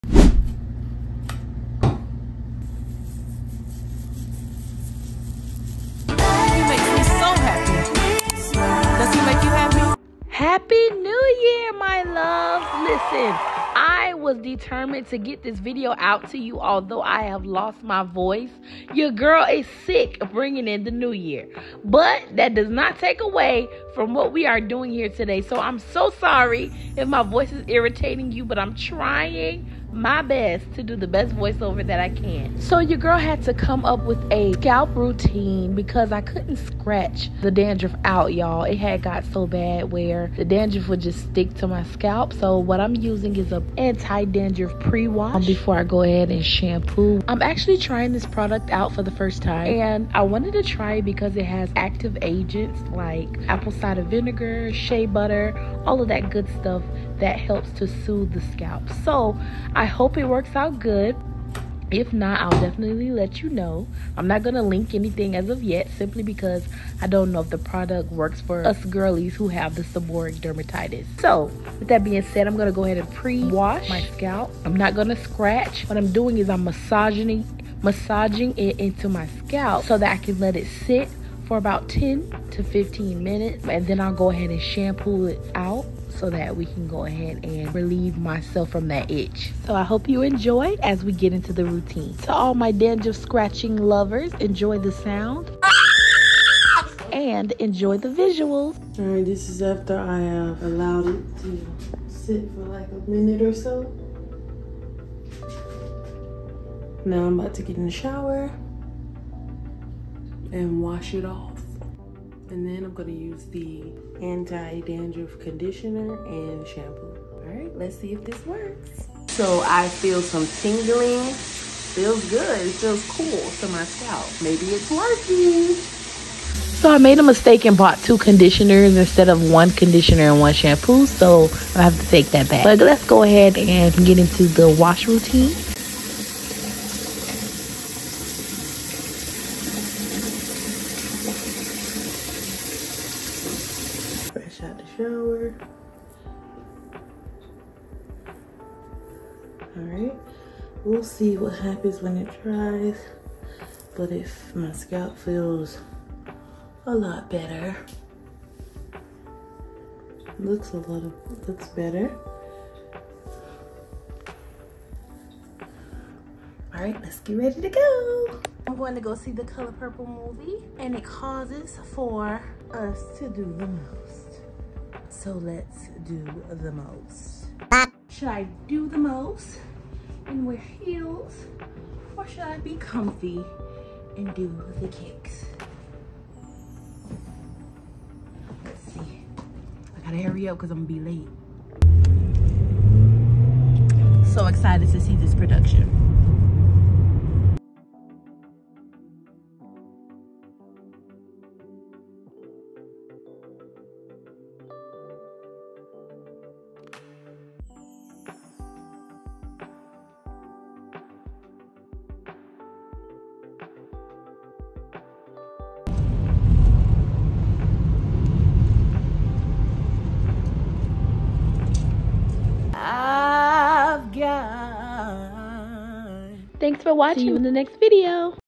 [NEW VIDEO🚨] ITCHY SCALP ROUTINE 🤕 + HAPPY NEW YEAR 🎆+ I SAW THE COLOR PURPLE MOVIE 🎥💜!
HAPPY NEW YEAR 🎆! Y’all I’m restarting my YouTube journey, trying to navigate how I create so that I’m not restricted with being compensated…

Besides that, I have missed y’all so much!!! Put in the comments what’s new?

I have so much to update you all on, I just don’t know where to start!

But I hope this video is a start!